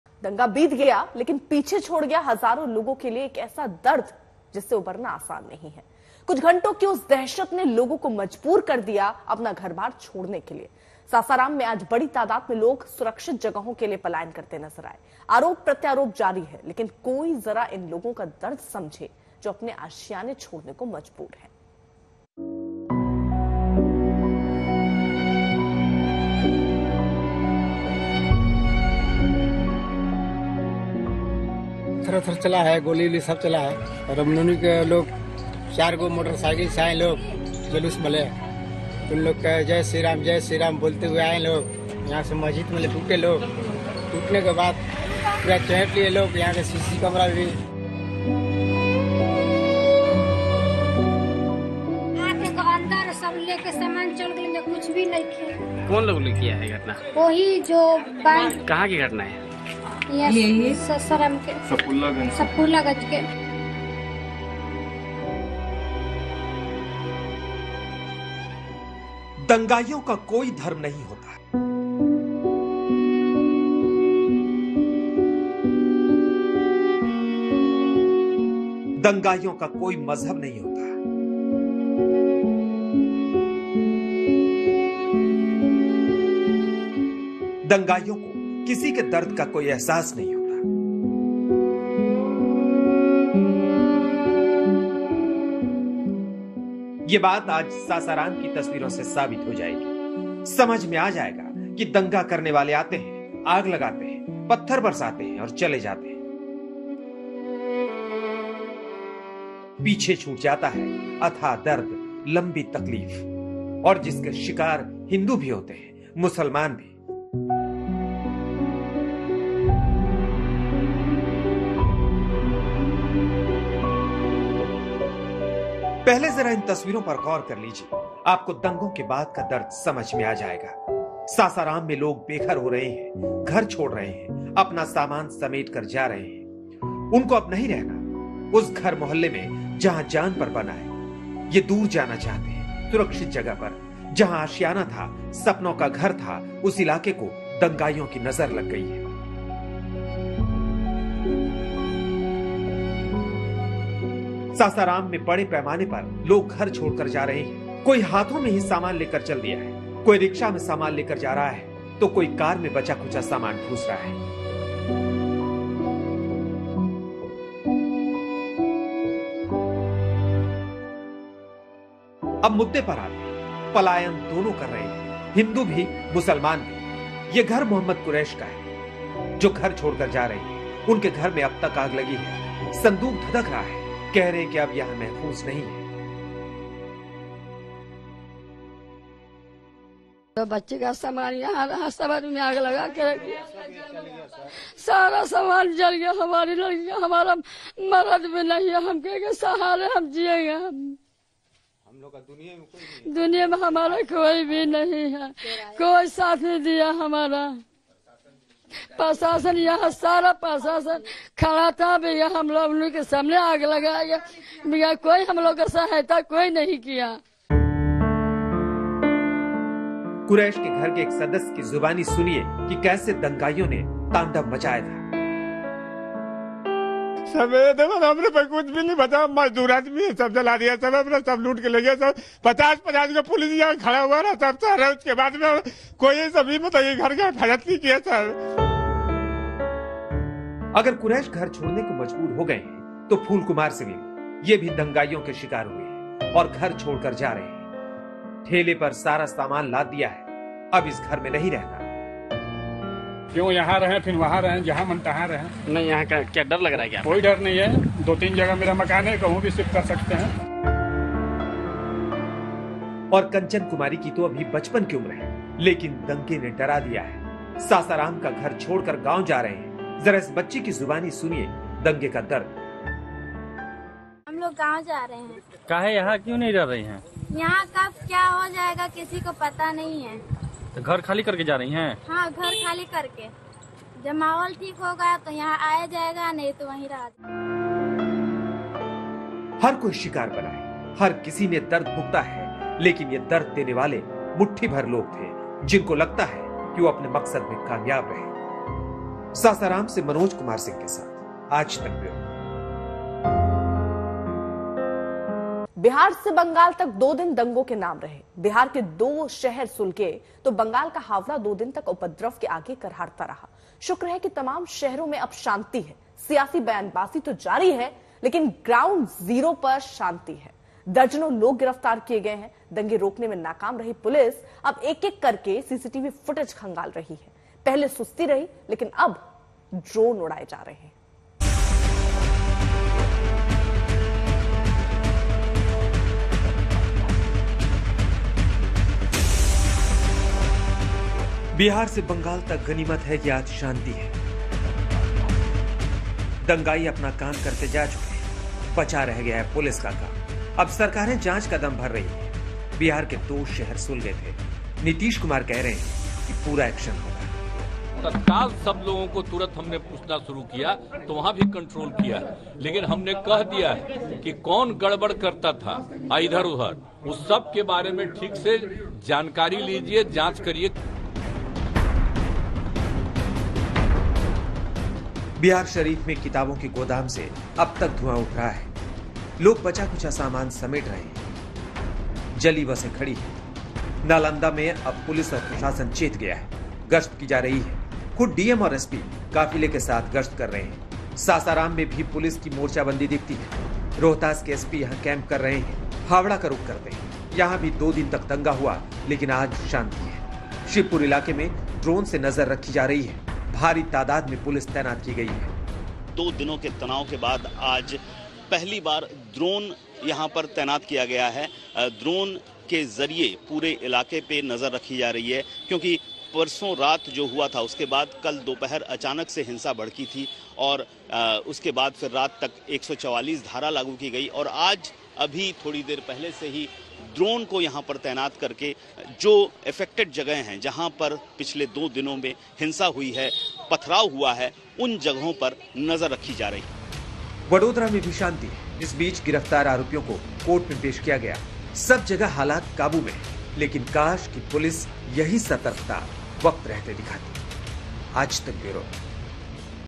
Sasaram में आज बड़ी तादाद में लोग सुरक्षित जगहों के लिए पलायन करते नजर आए | Bihar News | Aaj Tak
कुछ घंटों की उस दहशत ने लोगों को मजबूर कर दिया है अपना घर बार छोड़ने के लिए । सासाराम में आज बड़ी  तादाद में लोग सुरक्षित जगहों के लिए पलायन करते नजर आए। आरोप प्रत्यारोप जारी है लेकिन कोई जरा इन लोगों का दर्द समझे जो अपने आशियाने छोडने को मजबूर हो गए.

That panic of a few hours has forced people to leave their homes. A large number of people were seen migrating to safer places in Sasaram today. Allegations and counter-allegations are going on but someone should understand the pain of these people who were forced to leave their homes.